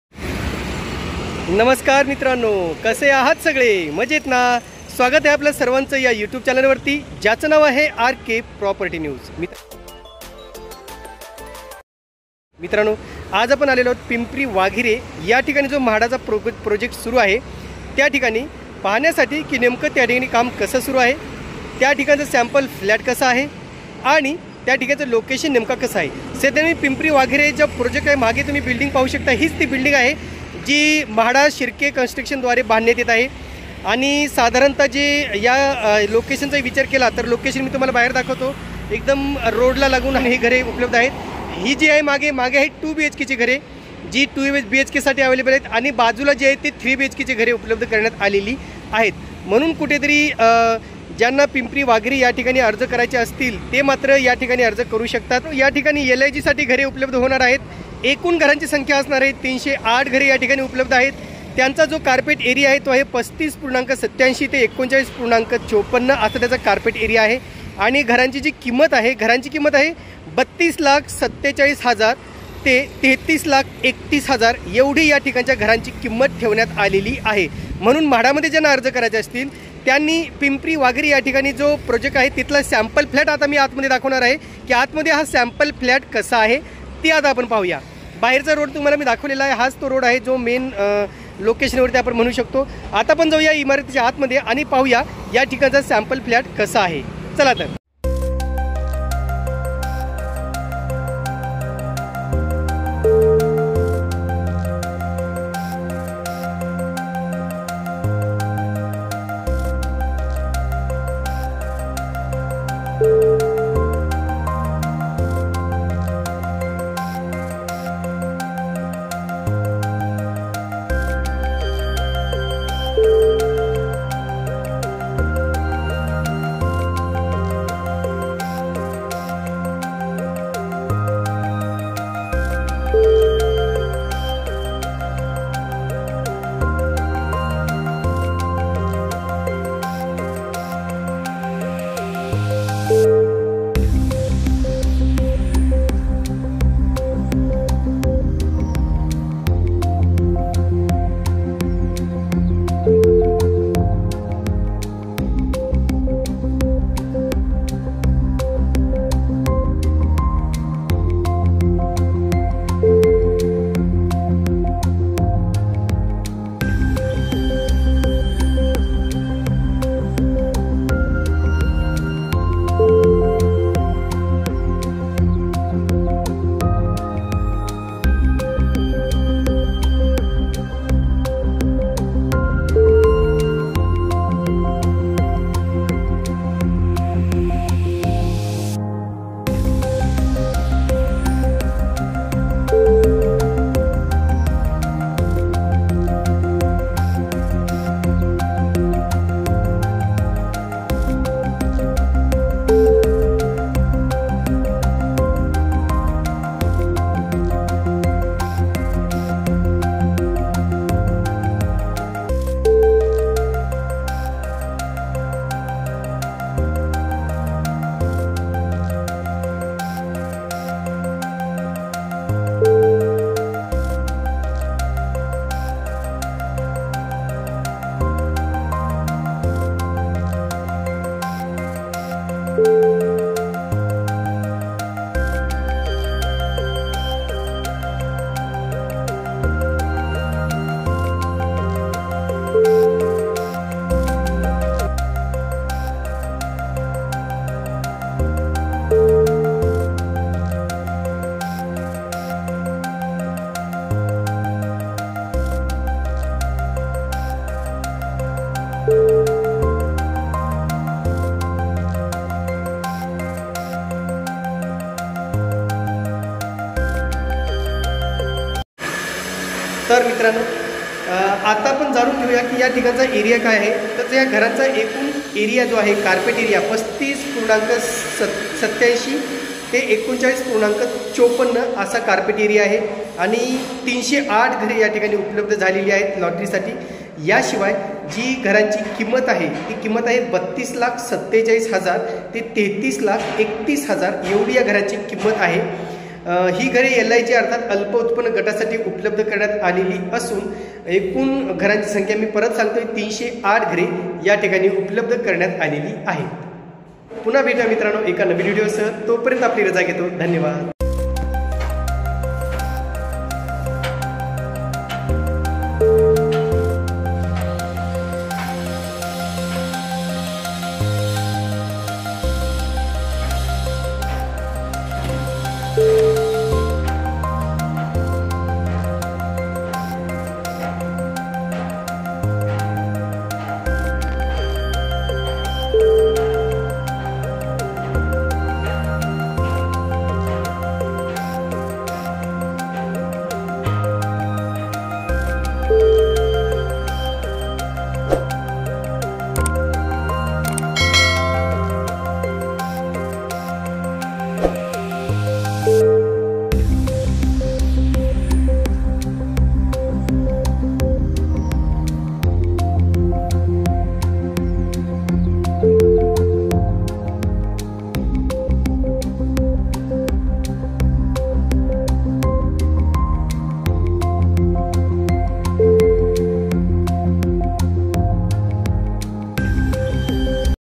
म्हाडा पुणे पिंपरी वाघिरे PCMC येथील प्रकल्पाचा 1 bhk सॅम्पल फ्लॅट. Mhada Pune Pimpari Waghere 1bhk.
म्हाडा पुणे पिंपरी वाघिरे PCMC येथील प्रकल्पाचा LIG 1 bhk सॅम्पल फ्लॅट. Mhada Pune Pimpari Waghere 1bhk.

आमचे इतर विडियो नक्की पहा.

सानपाडा रेल्वे स्थानक सिडको महागृहनिर्माण